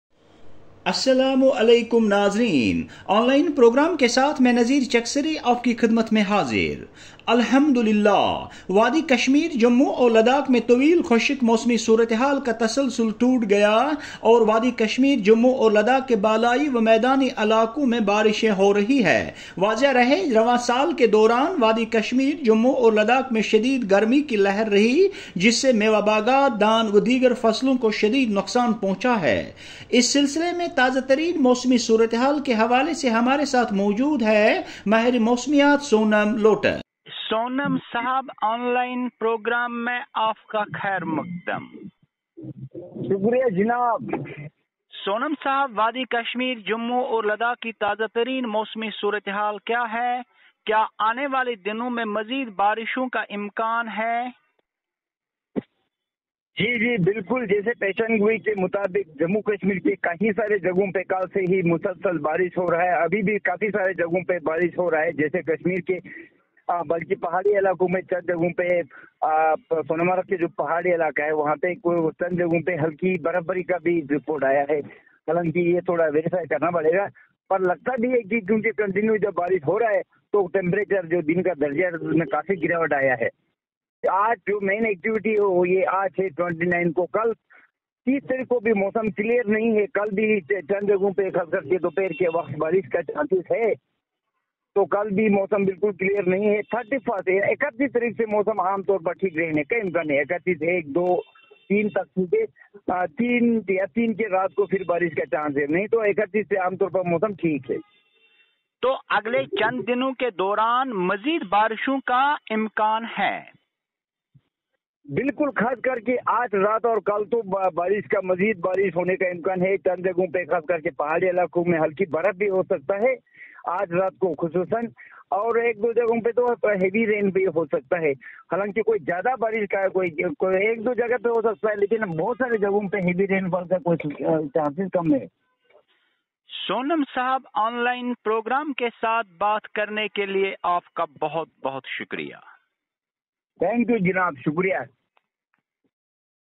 (𝗪𝗲𝗮𝘁𝗵𝗲𝗿 𝗨𝗽𝗱𝗮𝘁𝗲)|Meteorological Department Leh Head, Sonam Lotus Exclusive Talk with tameel irshad
(𝗪𝗲𝗮𝘁𝗵𝗲𝗿 𝗨𝗽𝗱𝗮𝘁𝗲) 𝟮𝟵/𝟬𝟴/𝟮𝟬𝟮𝟰 || Meteorological Department Leh Head, Sonam  Lotus Exclusive Talk with tameel irshad Regarding Weather Updates in J&K and Ladakh.